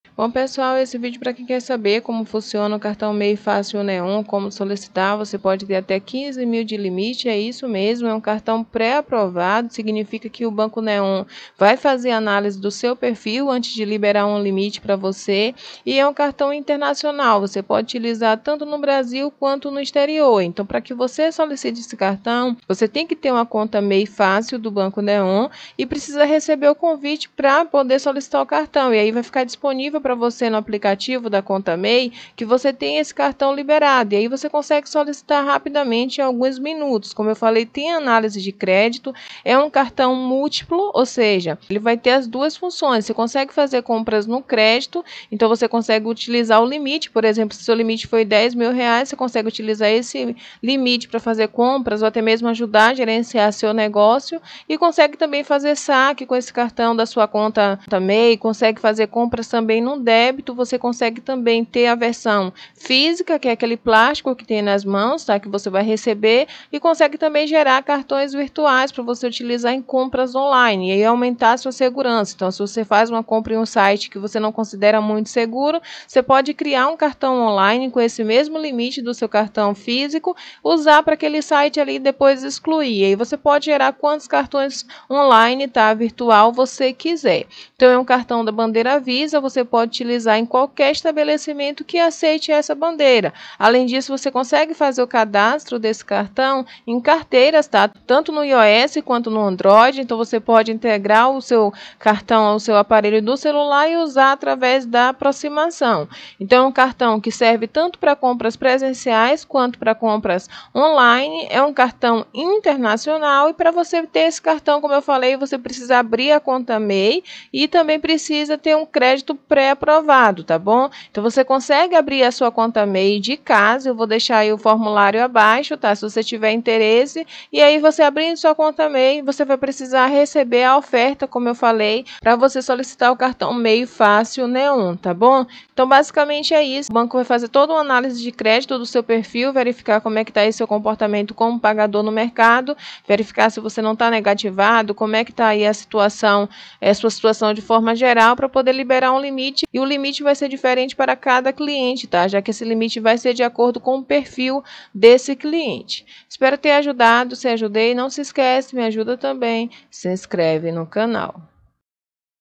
CARTÃO MEI FÁCIL NEON - Como funciona e cartão de crédito mei fácil neon
Nesse vídeo você saberá  como funciona  o cartão mei fácil neon , bem como  pedir o cartão de crédito mei fácil neon. Portanto, se você é microempreendedor individual e deseja ter acesso ao
cartão de credito mei neon assista ao vídeo .